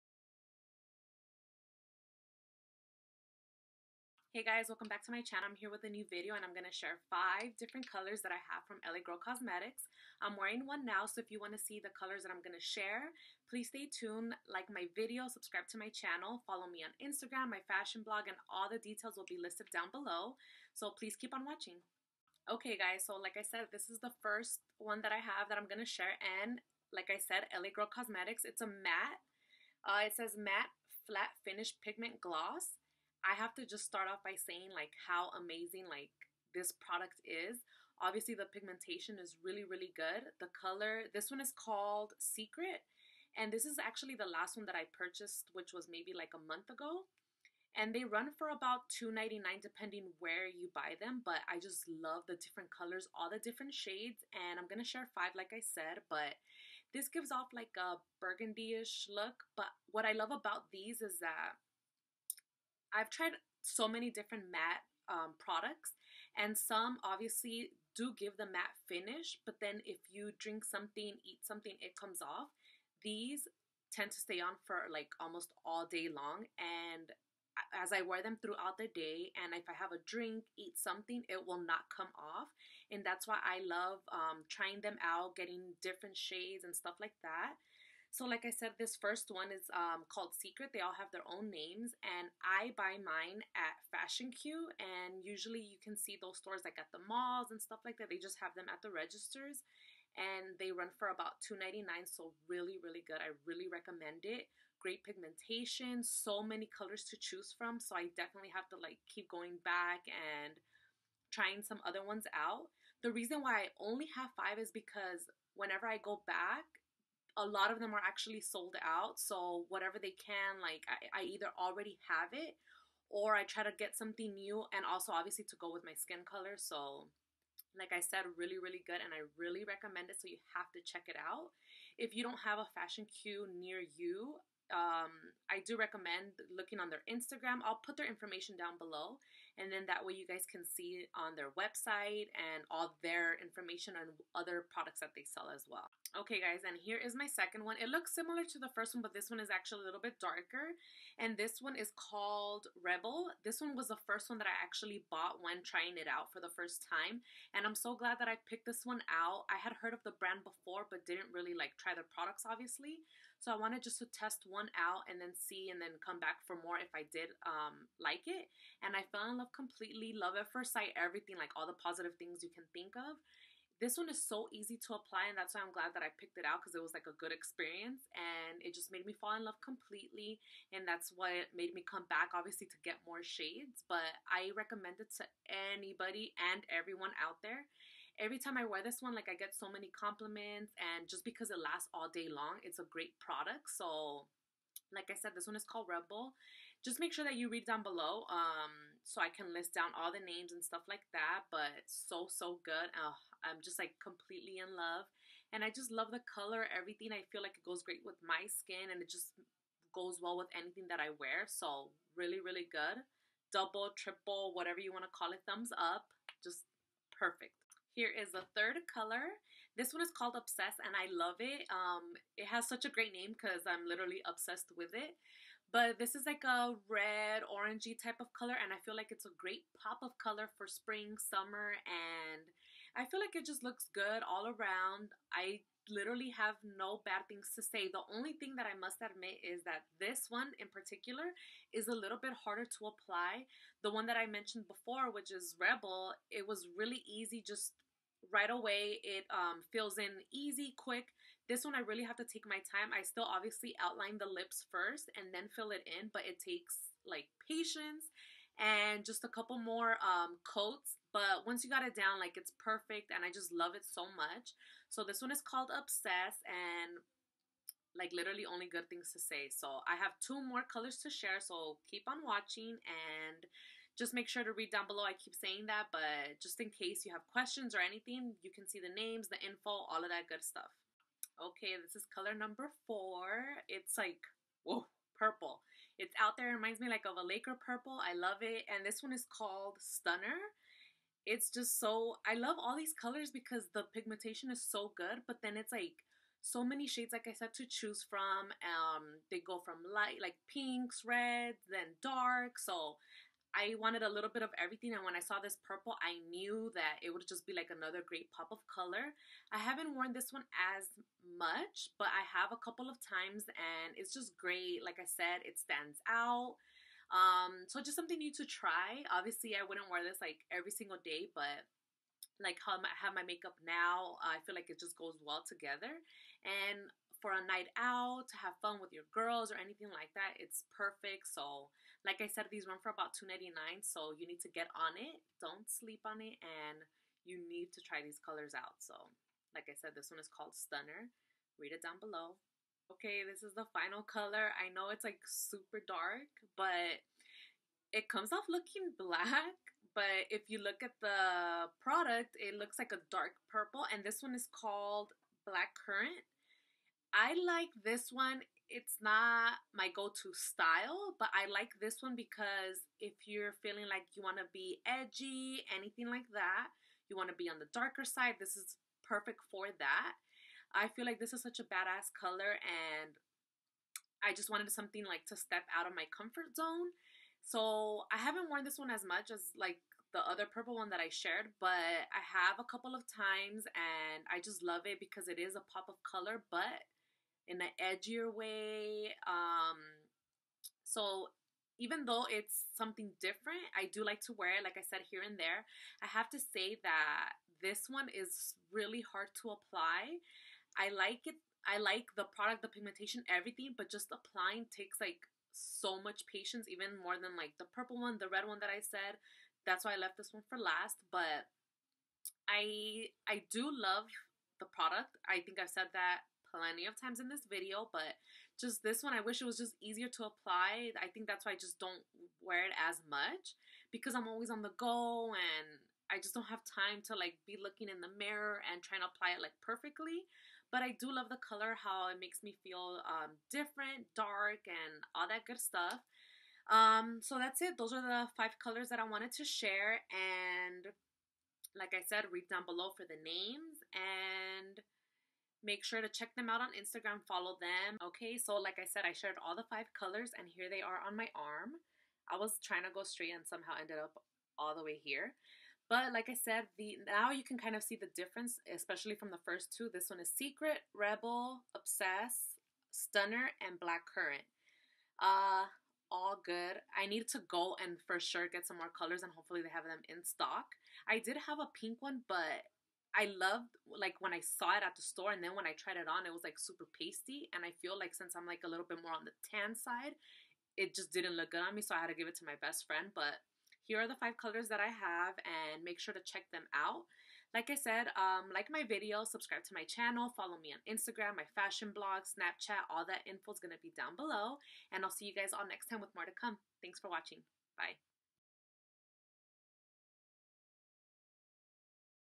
L.A. Girl Matte Lip Swatches & Review 2016
Hey guys, new video sharing 5 different matte shades.

Names in order
-Secret
-Rebel
-Obsess
-Stunner
-Black Currant

Follow them on Instagram @LAGIRLCOSMETICS

xoxo